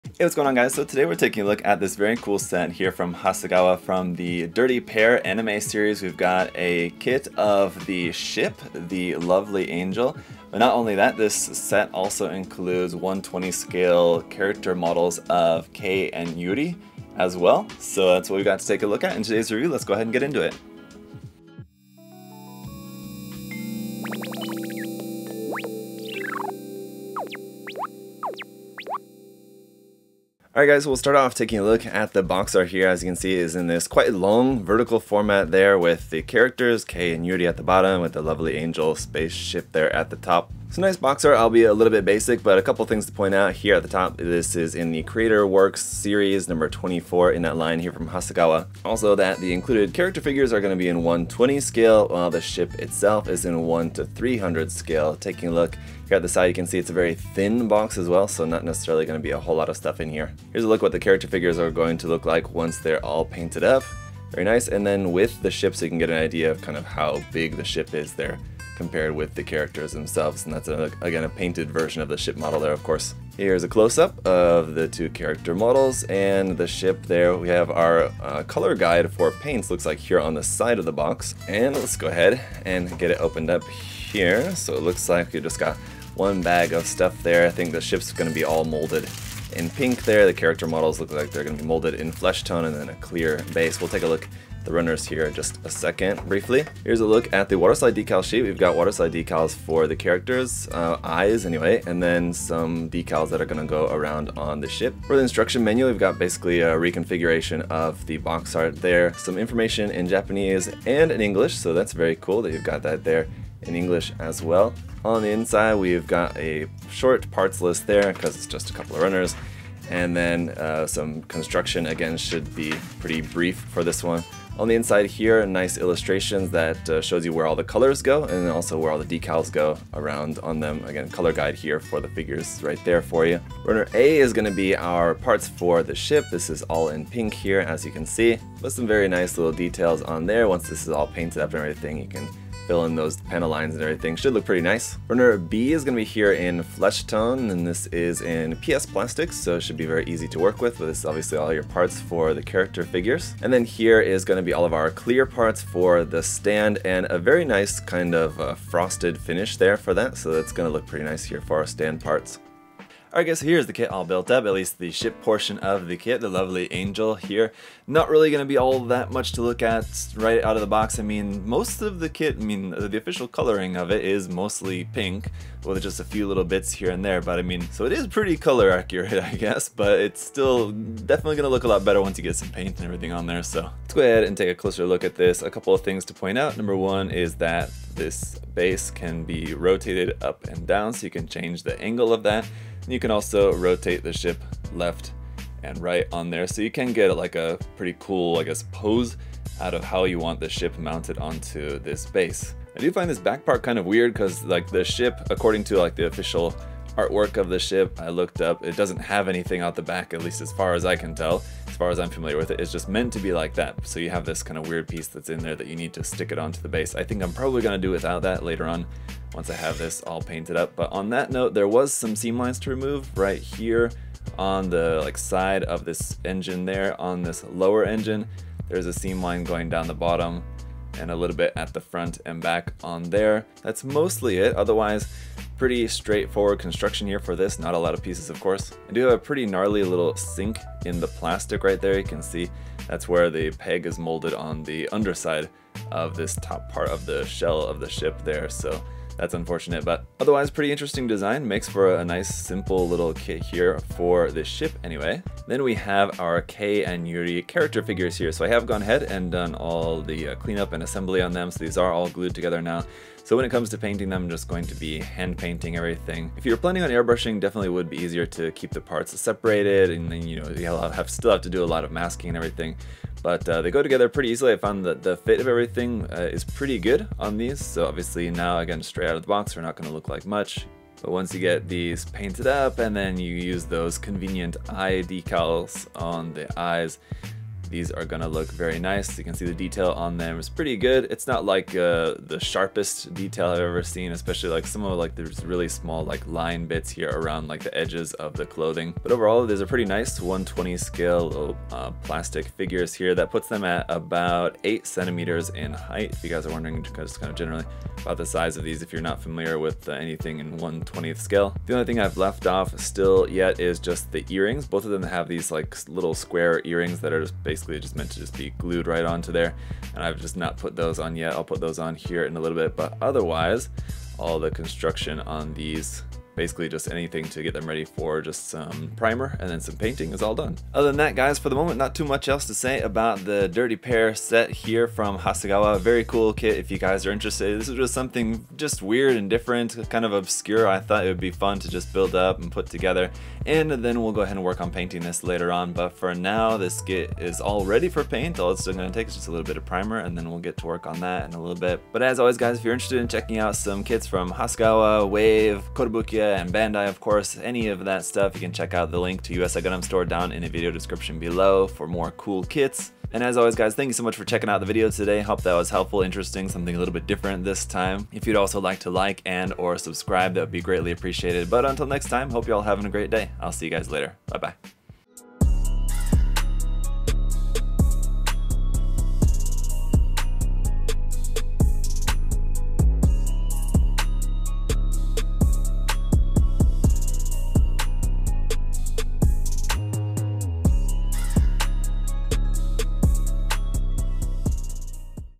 Dirty Pair Kei & Yuri + Lovely Angel - UNBOXING and Review!
New from Haseagaw - in this set we have Kei and Yuri in 1/20 scale and the Lovely Angel in 1/300 scale! From the classic anime "Dirty Pair" this set will be a lot of fun!